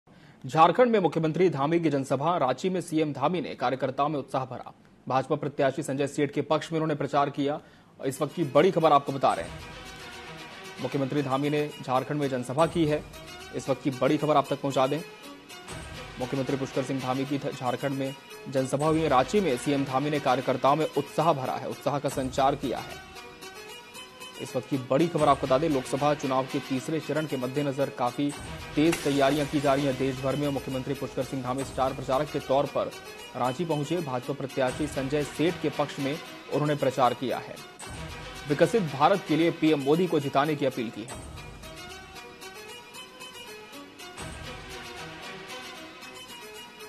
झारखंड में सीएम धामी की जनसभा, भाजपा प्रत्याशी संजय सेठ के पक्ष में किया प्रचार
Video Tilte :- झारखंड में सीएम धामी की जनसभ, भाजपा प्रत्याशी संजय सेठ के पक्ष में किया प्रचार